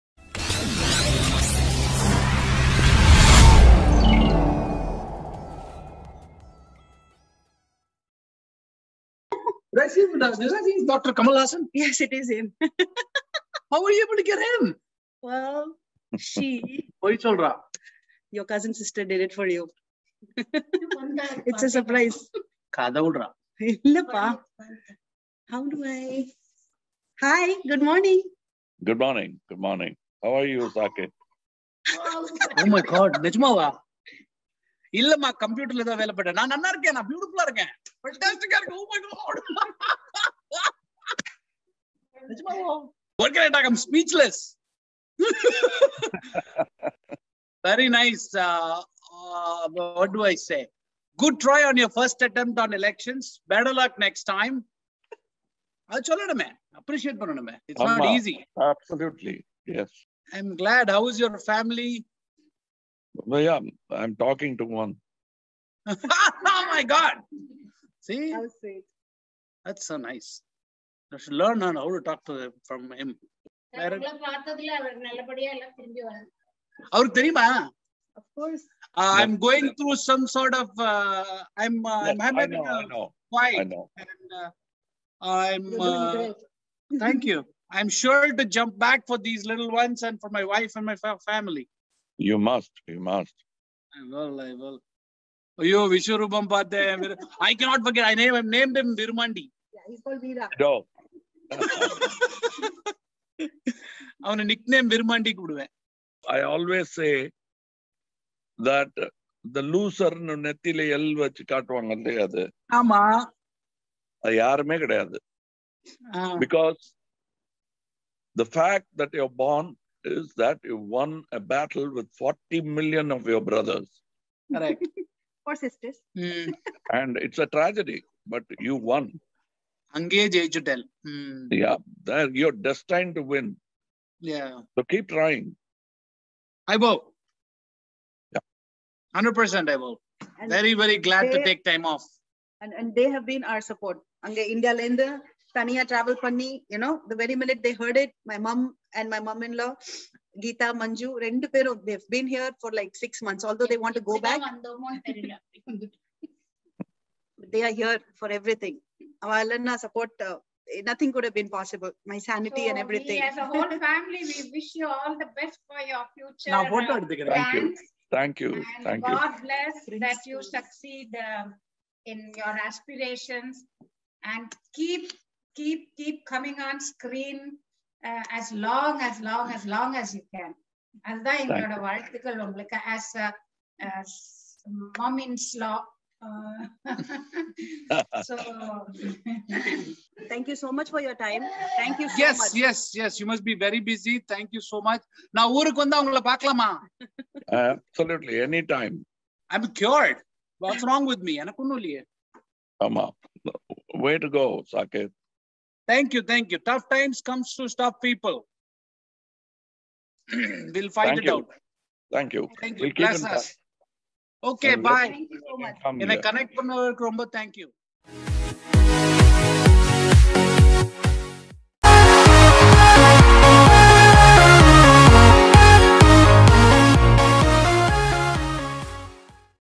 Mr.Kamal Haasan connected via zoom call to surprise his fan Mr. Saketh | Chennai
வணக்கம்,

கனடாவில் மூளை புற்றுநோயுடன் (Stage 3) போராடும் தனது தீவிர ரசிகரான திரு. சாகேத் அவர்களுடன் நம்மவர் திரு. கமல் ஹாசன் அவர்கள், zoom call மூலம் உரையாடினார்.

10 நிமிடங்களுக்கும் மேலாக நீண்ட உரையாடலில், அவருடனும் அவர் குடும்பத்துடனும் பேசிய நம்மவர், அவர் விரைவில் குணமடைய நம்பிக்கை வார்த்தைகளை அளித்தார்.

நன்றி.

Greetings,

Mr. Kamal Haasan connected via zoom call to surprise his fan Mr. Saketh who has been diagnosed with terminal brain cancer (stage 3).

He interacted with him and his family for more than 10 minutes and gave him words of encouragement to help him fight his illness.